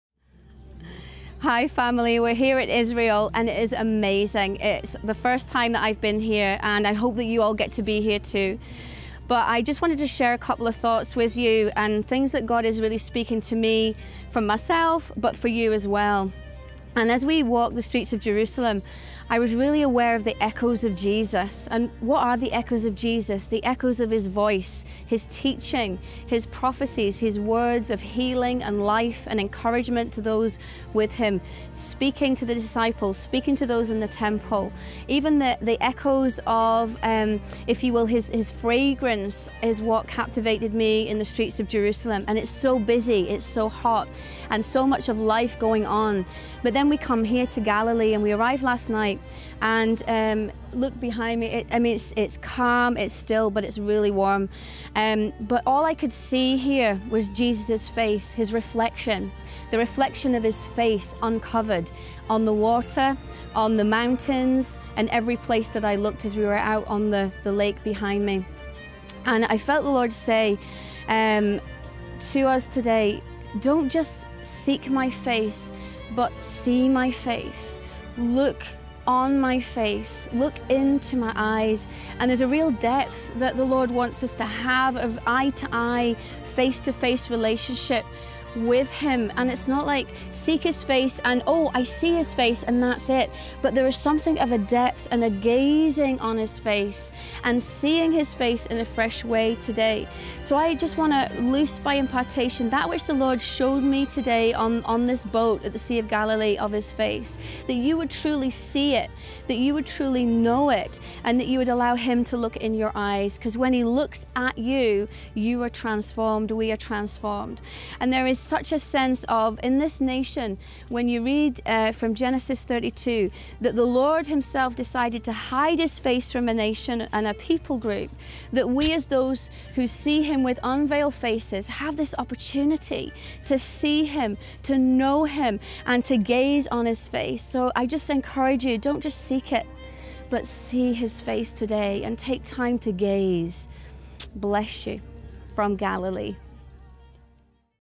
The Face of Jesus (Shortbread Prophetic Word) - Sarah-Jane Biggart, Glasgow Prophetic Centre
Sarah-Jane encourages us to press in to see the face of Jesus, in this shortbread message from the shores of the Sea of Galilee, in Israel.

This video is part of our Shortbread series- tasty, bite-size prophetic thoughts to encourage you, from the Glasgow Prophetic Team.